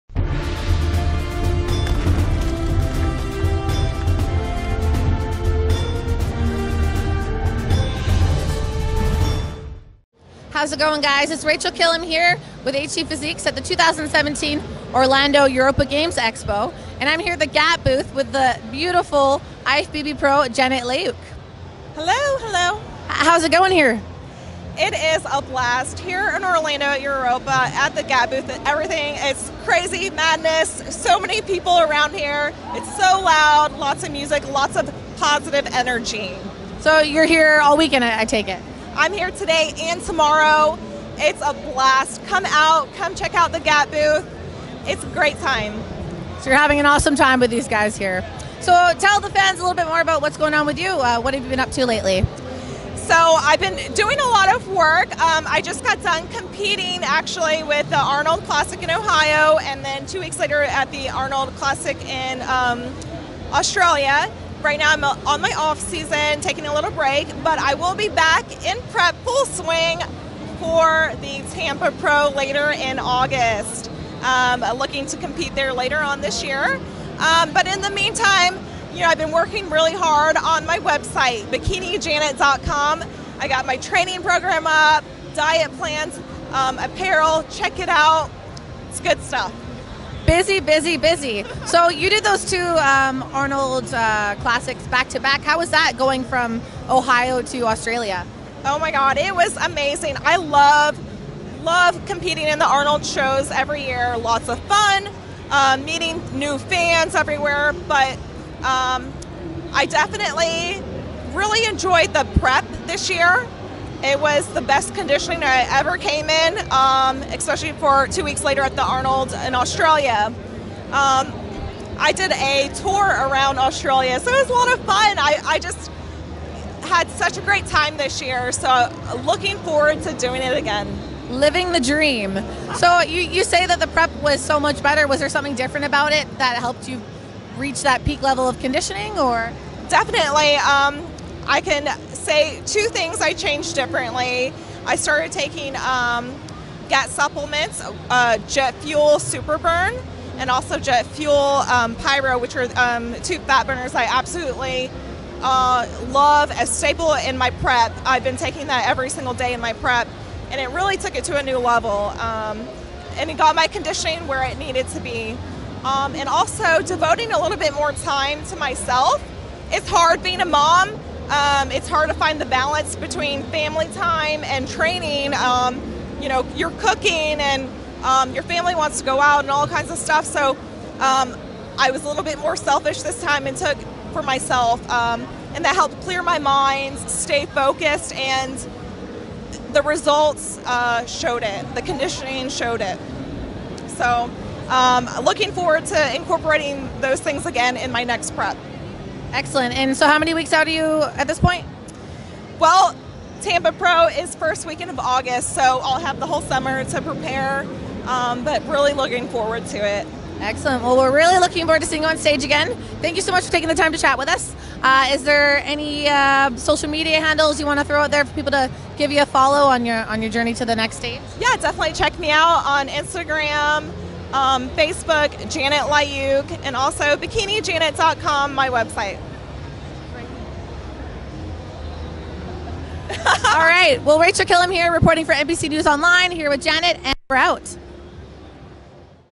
IFBB Bikini Pro Janet Layug Interviewed At The 2017 IFBB Orlando Europa
IFBB Bikini Pro Janet Layug Interviewed At The 2017 IFBB  Orlando Europa . Interview By Rachel Killam. npcnewsonline.com